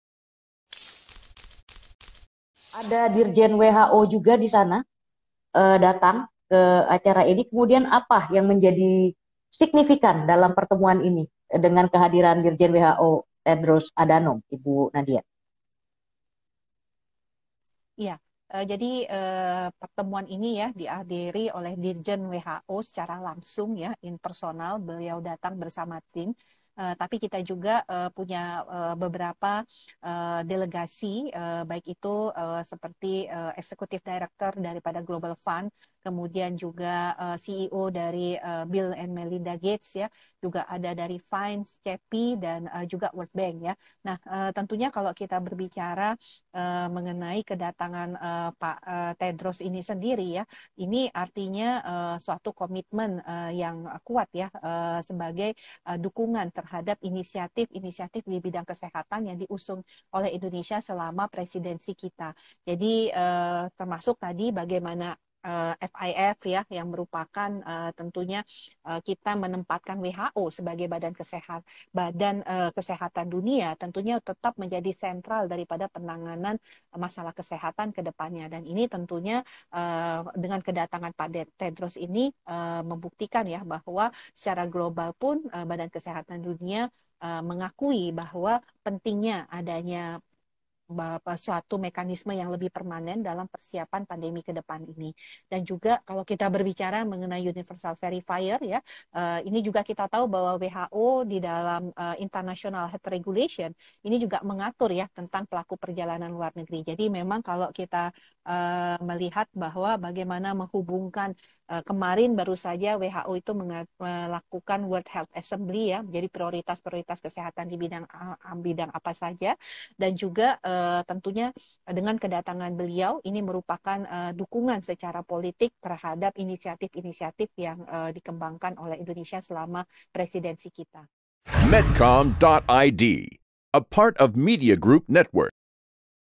Kehadiran Dirjen WHO ke DIY Bentuk Dukungan Terhadap Inisiatif RI di Bidang Kesehatan
Kementerian Kesehatan menggelar pertemuan pertama Menteri Kesehatan Negara Anggota G20 (The 1st G20 Health Ministers Meeting) di Daerah Istimewa Yogyakarta pada 20-21 Juni 2022. Pertemuan ini dihadiri langsung Dirjen WHO Tedros Adhanom Ghebreyesus. Kehadiran Dirjen WHO ini artinya dukungan terhadap inisiatif-inisiatif di bidang kesehatan yang diusung oleh Indonesia selama presidensi.